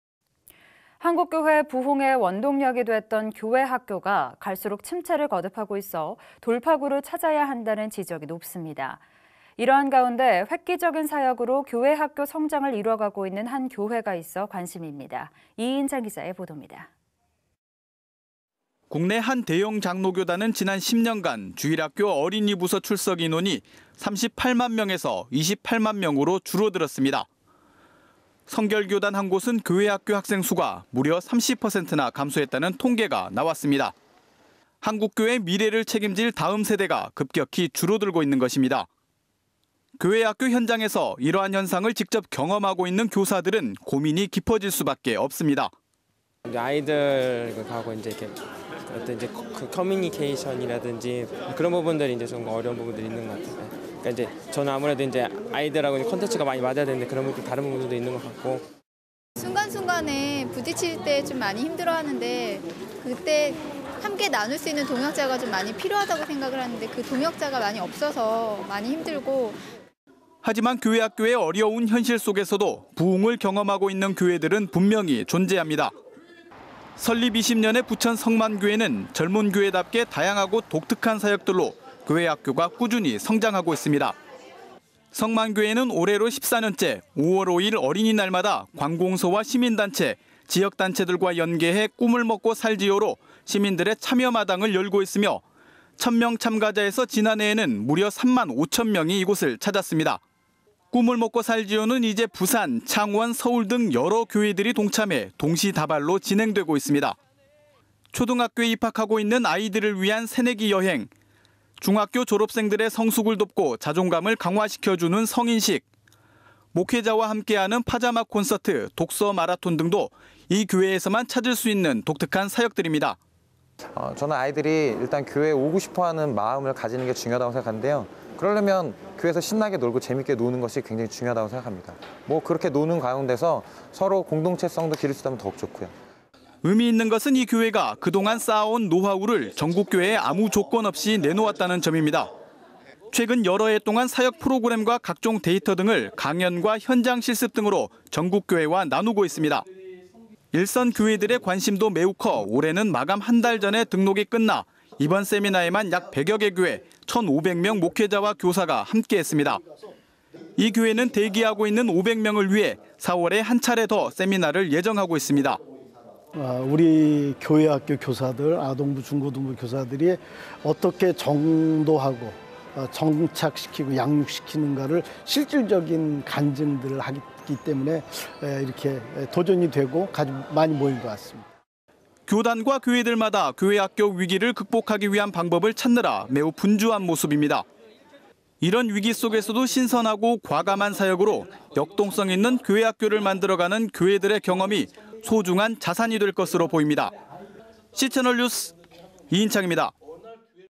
[C채널뉴스] 성만교회 '교회학교 노하우를 나눠요!'
한국교회 부흥의 원동력이 됐던 교회학교가 갈수록 침체를 거듭하고 있어 돌파구를 찾아야 한다는 지적이 높습니다. 이런 가운데 획기적인 사역으로 교회학교 성장을 이뤄가고 있는 한 교회가 있어 관심입니다. 이인창 기자의 보돕니다.

국내 한 대형 장로교단은 지난 10년간 주일학교 어린이 부서 출석인원이 38만명에서 28만명으로 줄어들었습니다. 성결교단 한 곳은 교회학교 학생수가 30%나 감소했다는 통계가 나왔습니다. 한국교회의 미래를 책임질 다음세대가 급격히 줄어들고 있는 겁니다.

교회학교 현장에서 이러한 현상을 직접 경험하고 있는 교사들은 고민이 깊어질 수 밖에 없습니다.

이선한 교사 / 광주반석교회 아동부
송영희 교사 / 영문교회 유치부

하지만 교회학교의 어려운 현실 속에서도 부흥을 경험하고 있는 교회들은 분명히 존재합니다. 설립 20년의 부천 성만교회는 젊은 교회답게 다양하고 독특한 사역들로 교회학교가 꾸준히 성장하고 있습니다.

성만교회는 올해로 14년째, 5월 5일 어린이날마다 관공서와 시민단체, 지역단체들과 연계해 '꿈을 먹고 살지요'로 시민들의 참여마당을 열고 있으며, 천명 참가자에서 지난해에는 무려 3만 5천명이 찾았습니다. '꿈을 먹고 살지요'는 이제 부산, 창원, 서울 등 여러 교회들이 동참해 동시다발로 진행되고 있습니다.

초등학교에 입학하는 아이들을 위한 '새내기 여행', 중학교 졸업생들의 성숙을 돕고 자존감을 강화시켜주는 '성인식', 목회자와 함께하는 '파자마 콘서트', '독서마라톤' 등도 이 교회에서만 찾을 수 있는 사역들입니다.

배상환 교사 / 성만교회 중고등부

의미있는 것은 이 교회가 그동안 쌓아온 노하우를 전국 교회에 아무 조건없이 내놓았다는 점입니다. 최근 여러해 동안 사역 프로그램과 각종 데이터 등을 강연과 현장실습 등으로 전국교회와 나누고 있습니다.

일선 교회들의 관심도 매우 커 올해는 마감 한달 전에 등록이 마감돼 이번 세미나에만 약 100개 교회, 1500명 목회자와 교사가 함께했습니다. 이 교회는 대기하고 있는 5백명을 위해 4월에 한 차례 더 세미나를 예정하고 있습니다.

이찬용 담임목사 / 성만교회

교단과 교회들마다 교회학교 위기를 극복하기 위한 방법을 찾느라 분주한 모습입니다. 이런 위기 속에서도 신선하고 과감한 사역으로 역동성있는 교회학교를 만들어가는 교회들의 경험이 소중한 자산이 될 것으로 보입니다. C채널 뉴스 이인창입니다.